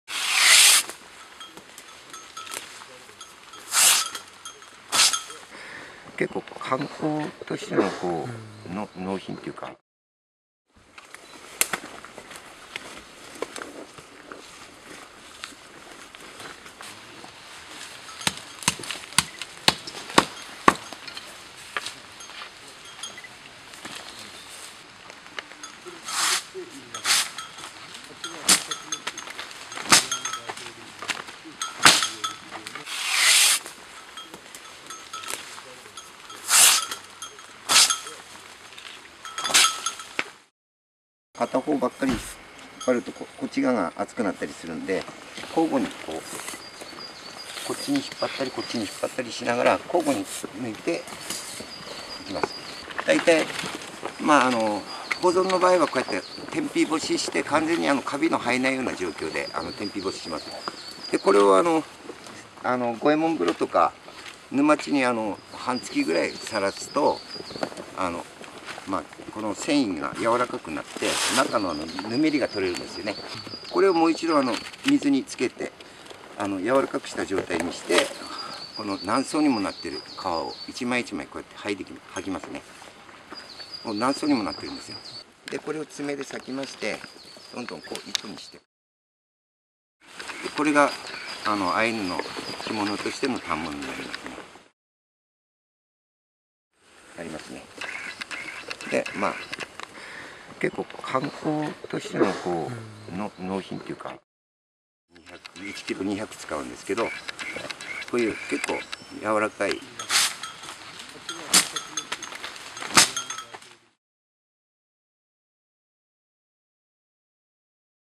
伝統工芸用の樹皮を採取 北海道でアイヌ民族 Ainu craftsmen collect tree bark for traditional cloth
北海道当麻町で１７日、アイヌ民族の伝統工芸の職人らが、織物「二風谷（にぶたに）アットゥシ」の原料になる樹皮を採集した。

Indigenous Ainu craftsmen collect tree bark to create cloth used in traditional dress on June 17, 2014, in Toma, Hokkaido.